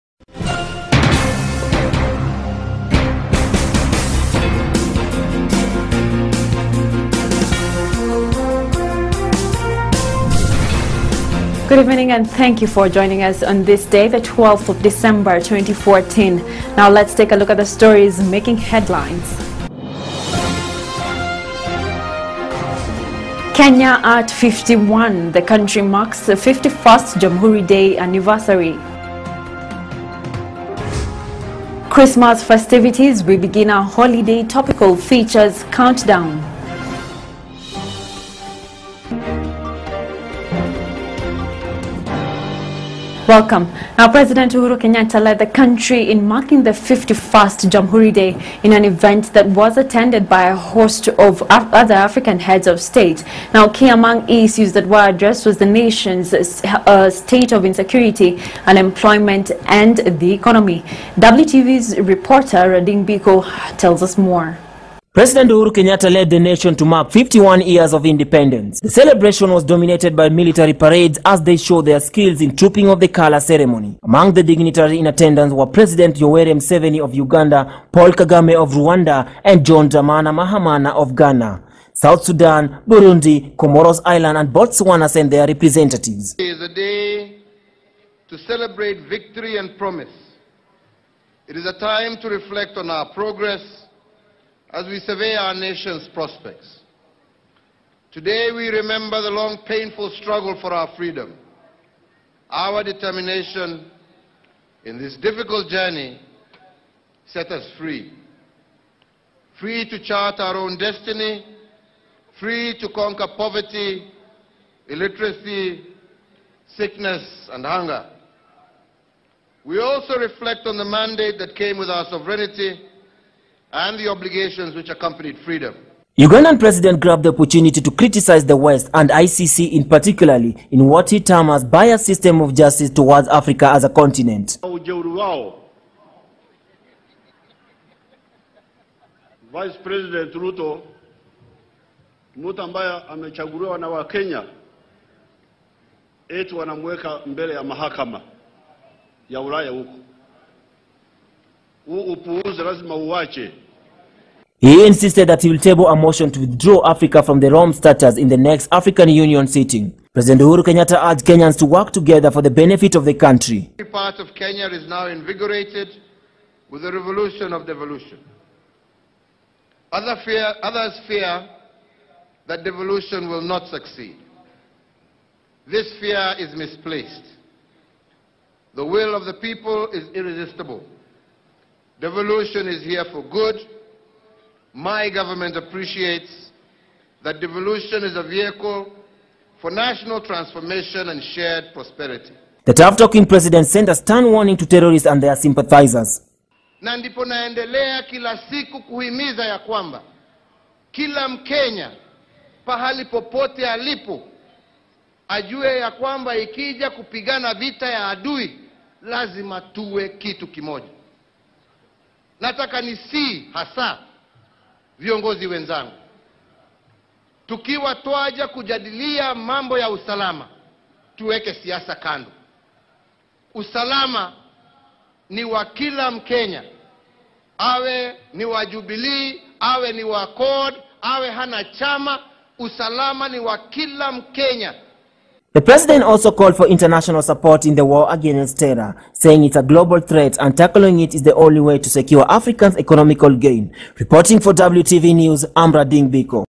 KENYA AT 51
President Uhuru Kenyatta led the country in marking the 51st Jamuhuri Day in an event attended by a host of other African heads state and government. Key among issues addressed were the nation’s state of insecurity, unemployment and the economy.